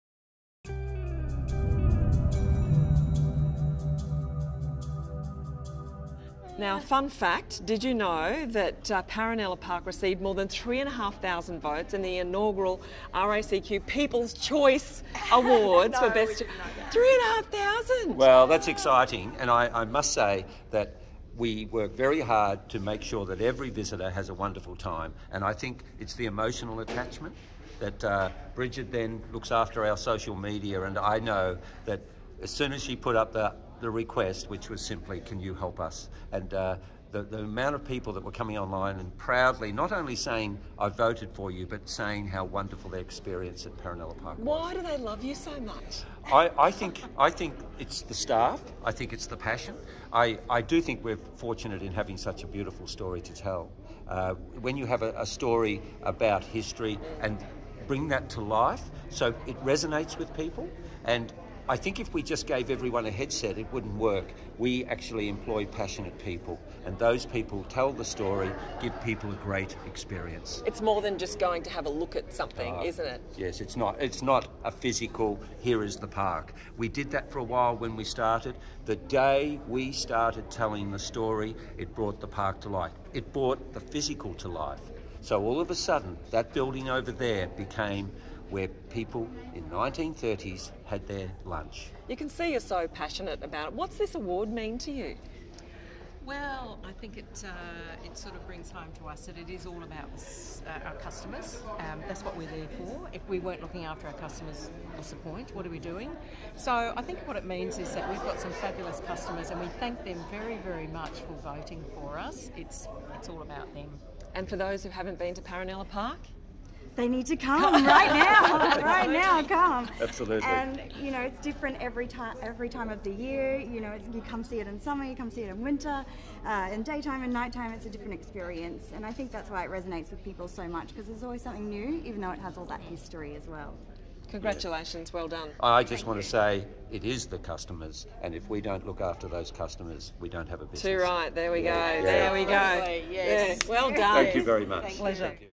Tourist Attraction Gold Winner: Paronella Park
Paronella Park in Tropical North Queensland took out gold in the Tourist Attraction Category at the 2017 Queensland Tourism Awards